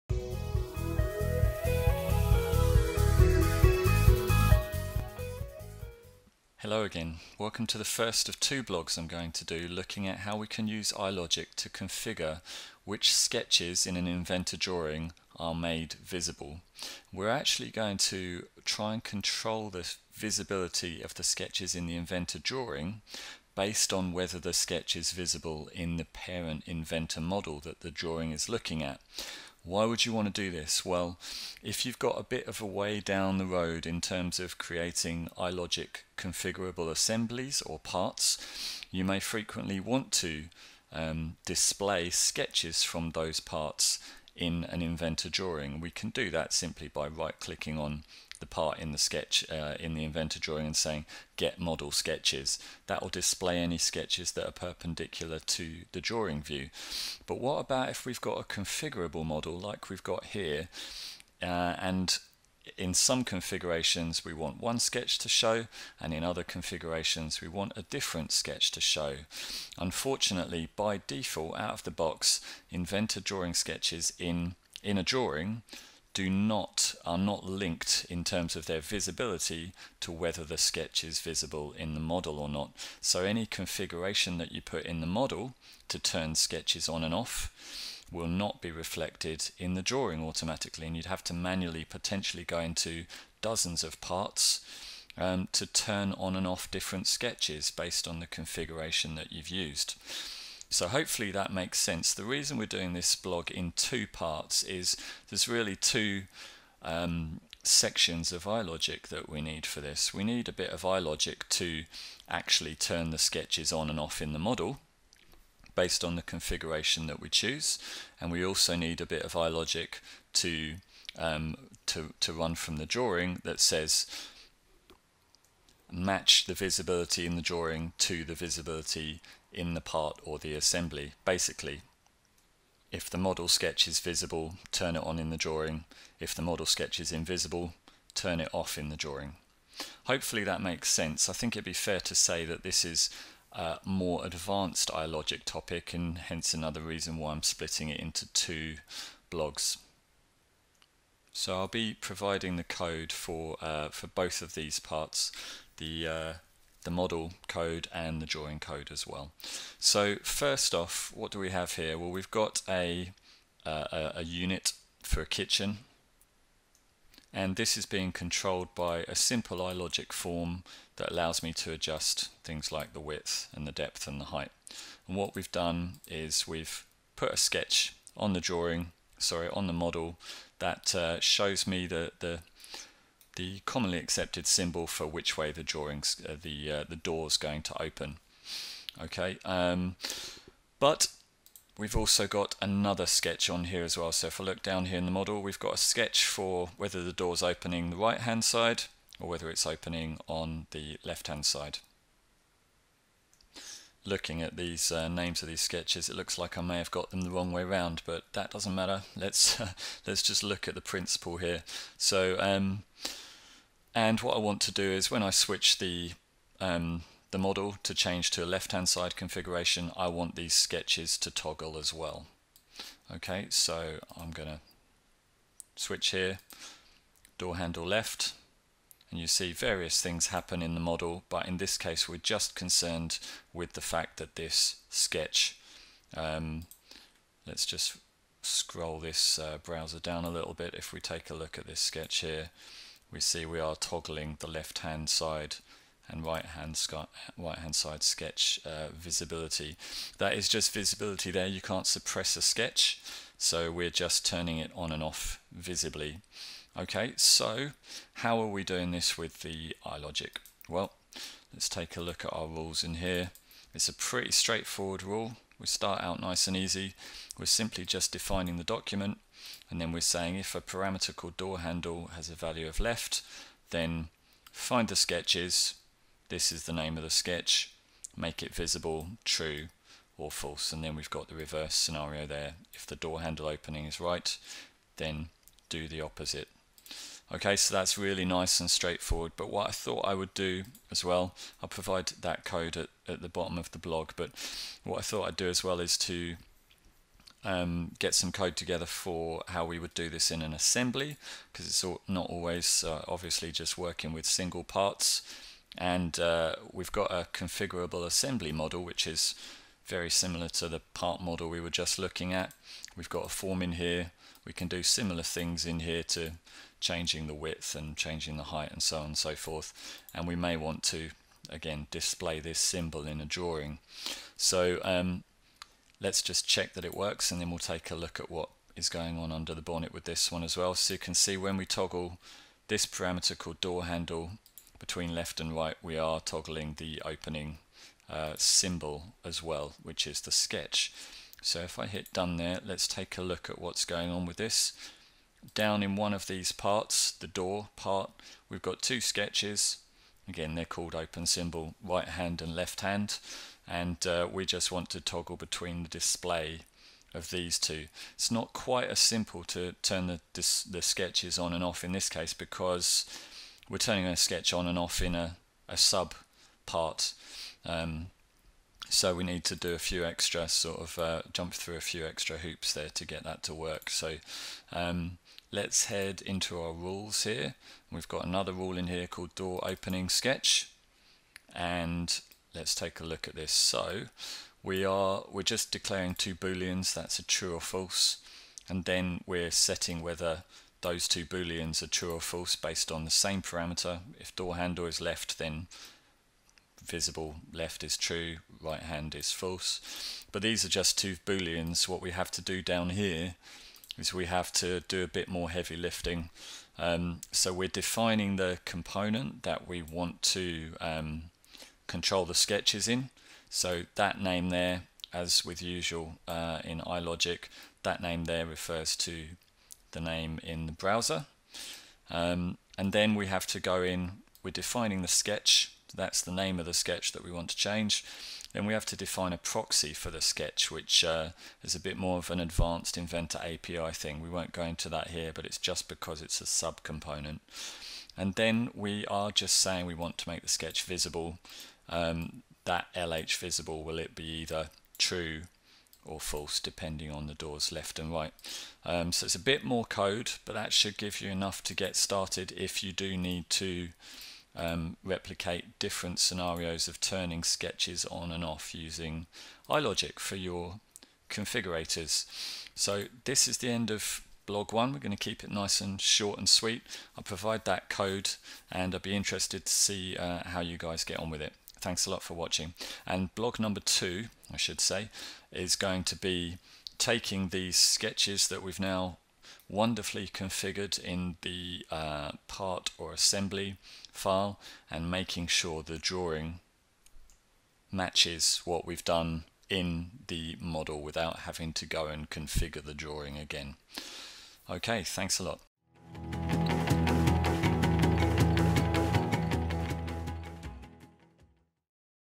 Autodesk Inventor 2018 - iLogic - Control Model Sketch Visibility
When producing iLogic configurator models, you may occasionally need to control the visibility of sketches as part of the configuration. Why would you want to do that I hear you ask? Well your sketches may need displaying downstream in a drawing view for instance, and you might want different sketches to be visible for different configurations of the design.

Note: Always think beforehand about whether you can use a single sketch instead of multiple sketches, then there will be no need to switch between them. In the video example, it may be more efficient to simply include multiple symbols in the same sketch, and control their position with parameters.